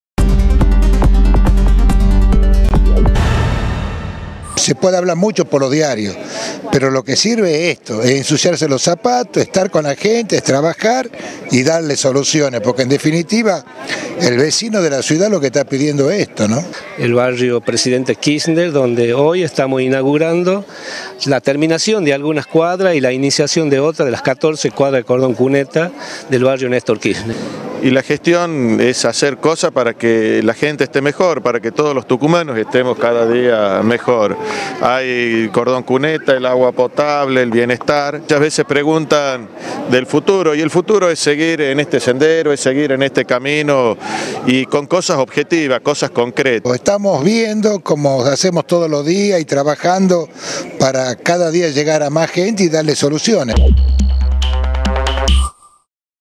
Alperovich: “A la gente hay que darle soluciones, es el pedido de los vecinos”- Tucumán
El Gobernador José Alperovich, realizó estas declaraciones luego de recorrer el Barrio Pte. Kirchner para dejar inaugurada cuadras de cordón cuneta que se ejecutan a través del Plan Más Cerca II. Allí destacó que el objetivo es seguir trabajando para que “Tucumán no vuelva a ser el de antes”.
"Estamos trabajando como todos los días. Lo que sirve es ensuciarse los zapatos para estar con la gente y darles soluciones, porque en definitiva, lo que pide el vecino de la ciudad es esto”, manifestó el primer mandatario al hacer referencia a la transformación positiva que vivió el Bº Pte. Kirchner que antes era un asentamiento y que actualmente cuenta con cuadras de cordón cuneta, agua potable, se tramita la titularización dominial de las viviendas, se edifican módulos habitacionales y pronto se comenzará con la construcción del sistema cloacal.